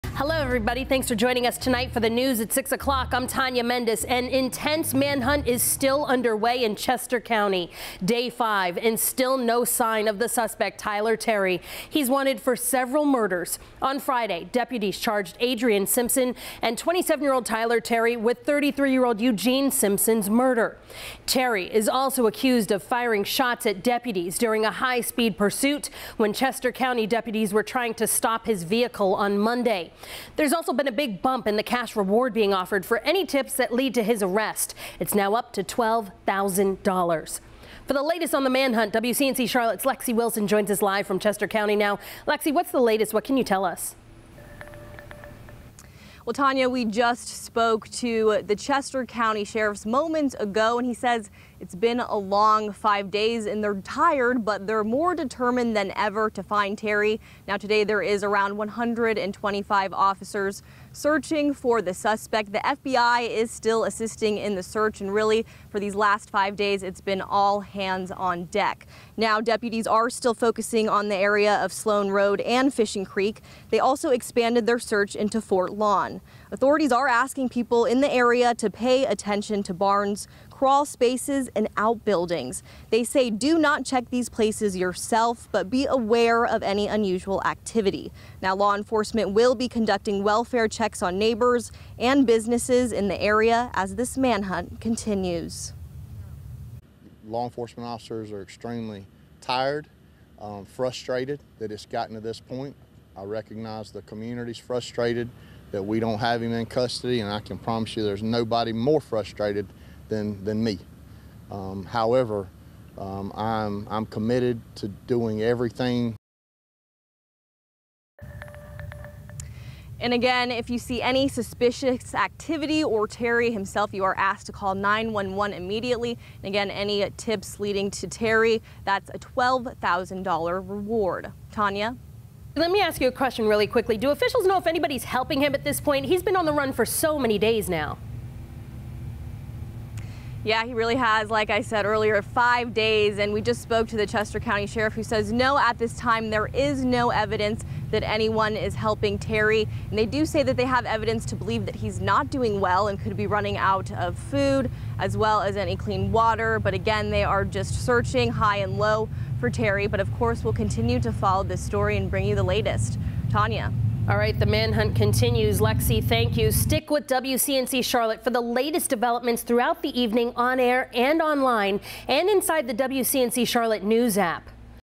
The hunt for Tyler Terry continues
FBI agents from Charlotte have been sent to Chester County, South Carolina, to assist authorities as they look for Terry. He has been on the run since late Monday after a police chase ended with a crash on Highway 9 near Richburg. Terry is accused of firing multiple shots at Chester County deputies during the chase, striking multiple vehicles.

The driver of the car, identified as Adrienne Simpson, was arrested at the scene of the crash and faces multiple charges. There is a $12,000 cash reward for any tip that leads to the capture of Terry.